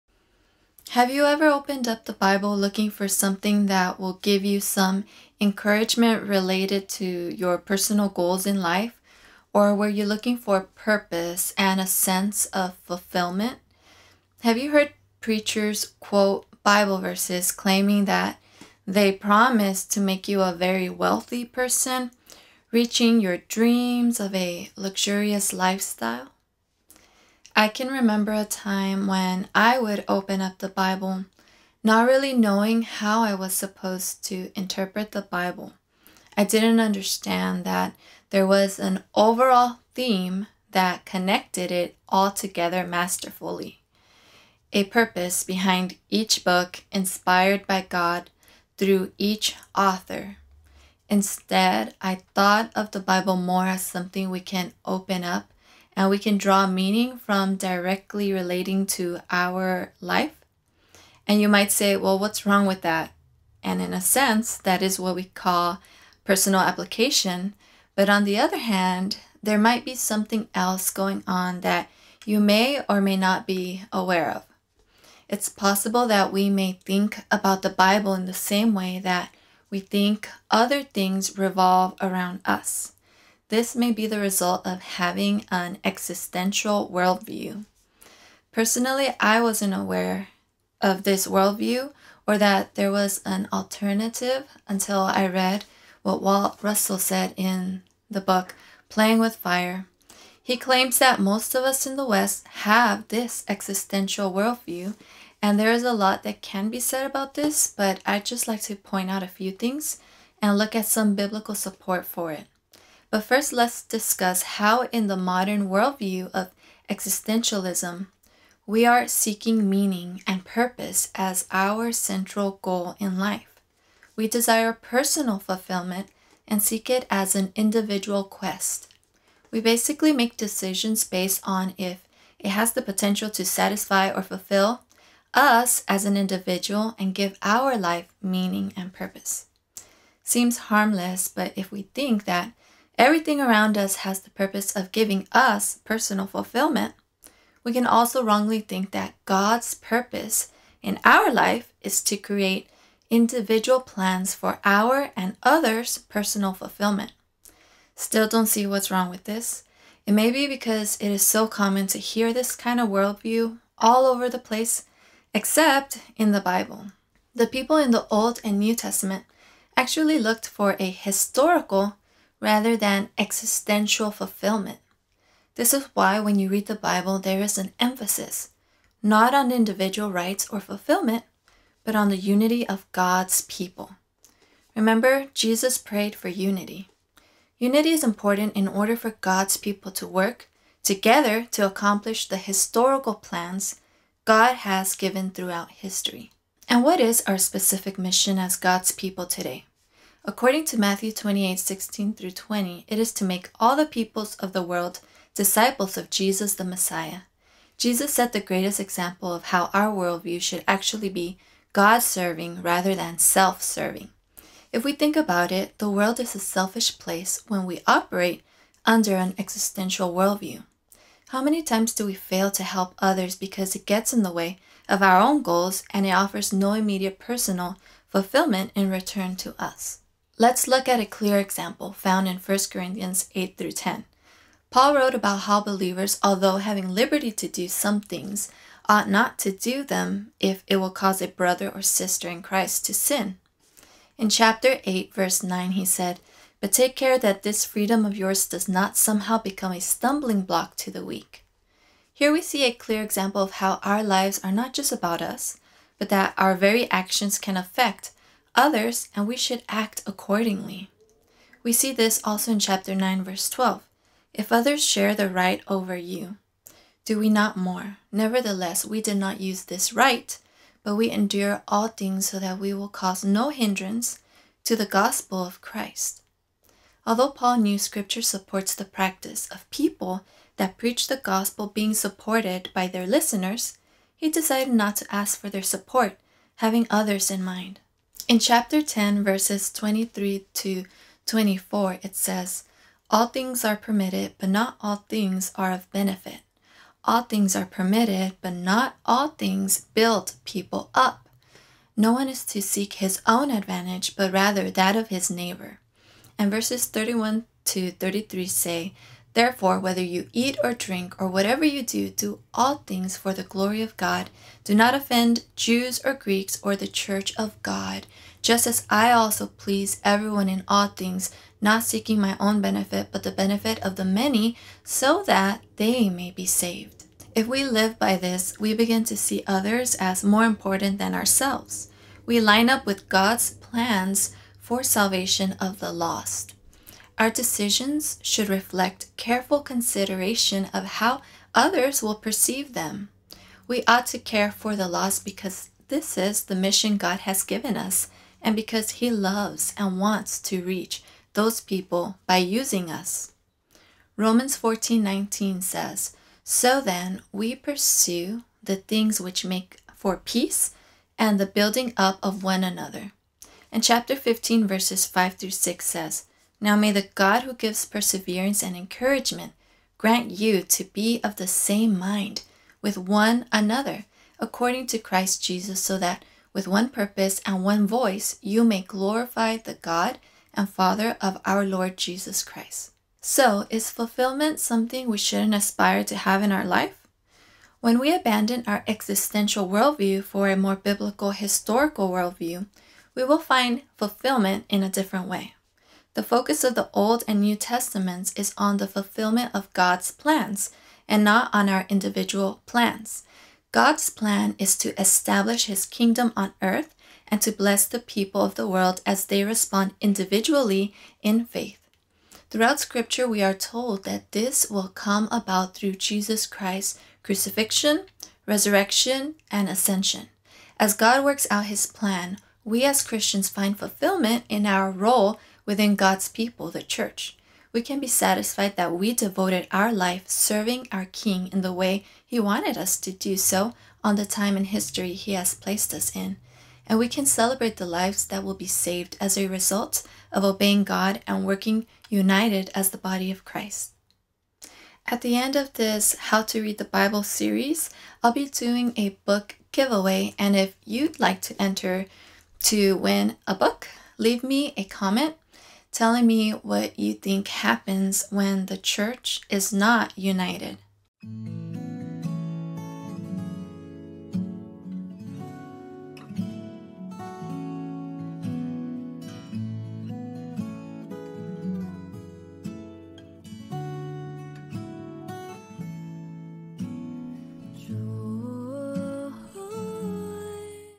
Existential Worldview VS Biblical Worldview | Part 3
Are you bringing an existential worldview when you read the Bible?  What does the Bible tell us our worldview should look like and what should fulfill us in this life?  I share some of my own thoughts on this video as well as charts and some paraphrased quotes from the book, "Playing With Fire" by Walt Russel

Enter my free book giveaway by answering the question at the end of this video in the comments section!